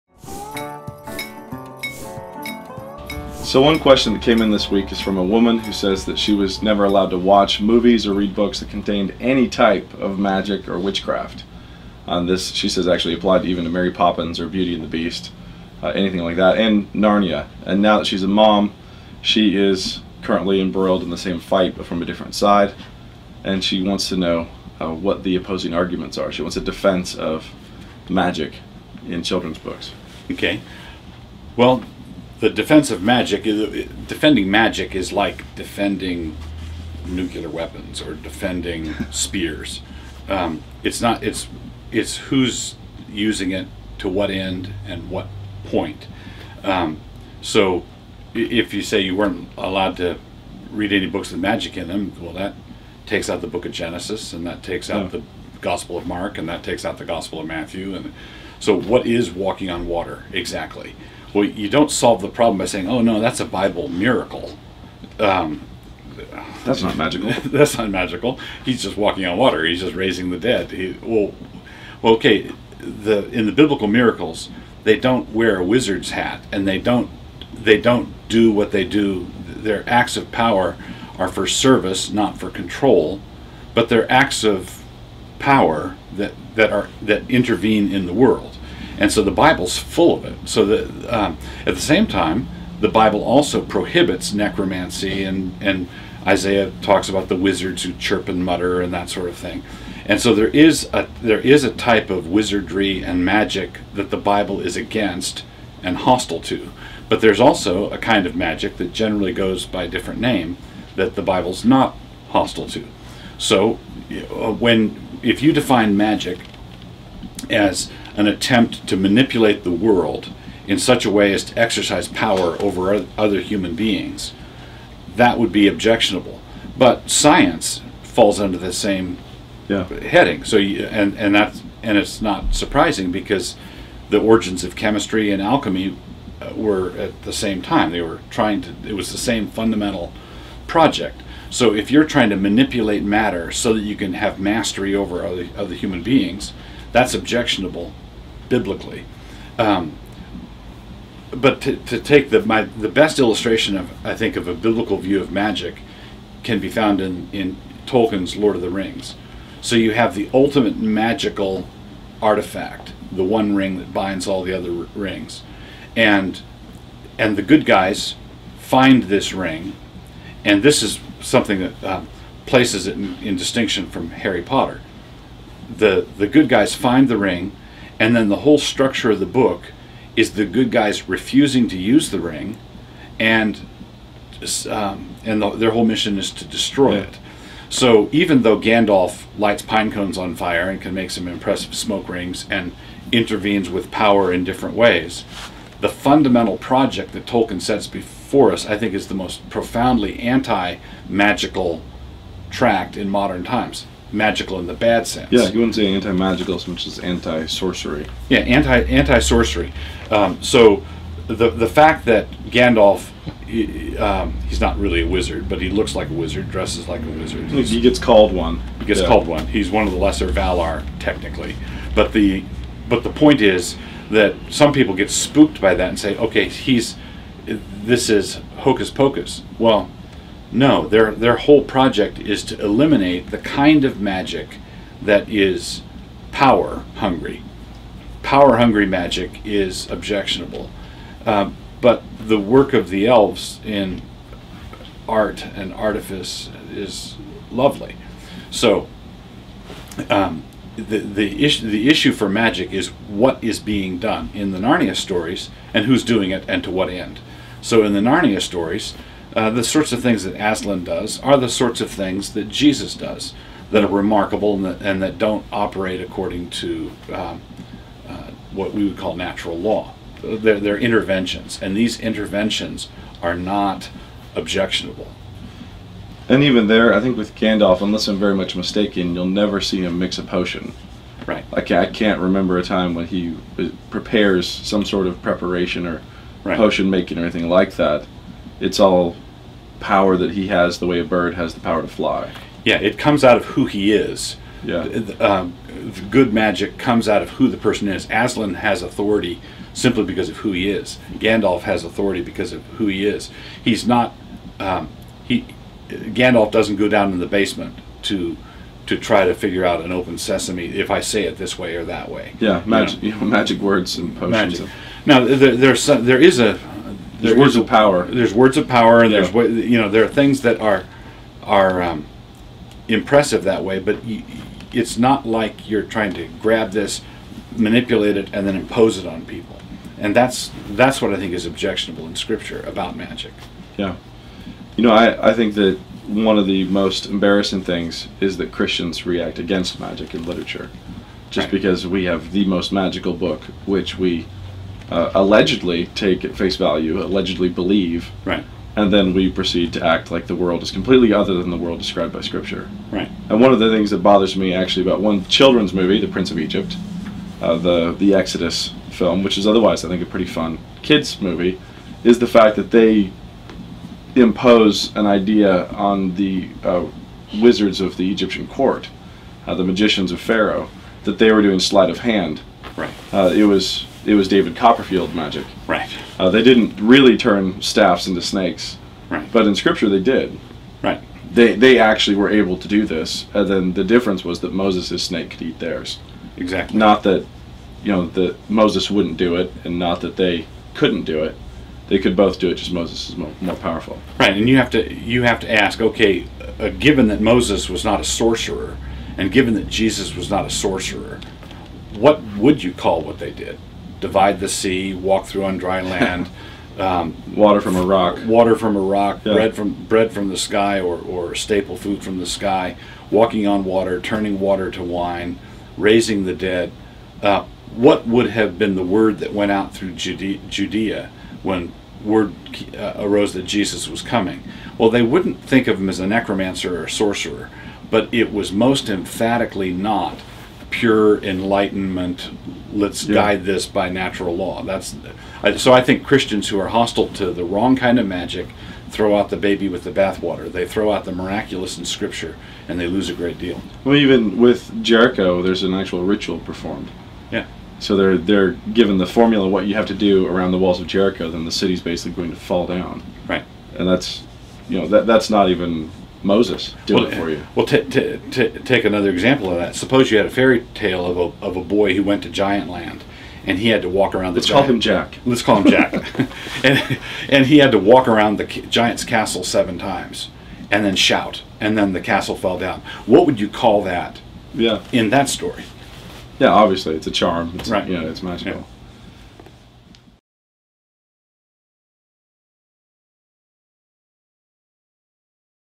Doug Wilson and N.D. Wilson talk Magic in Literature Part 1
N.D. WIlson and Doug Wilson discuss whether Christians parents encourage their kids to read books with Magic in them?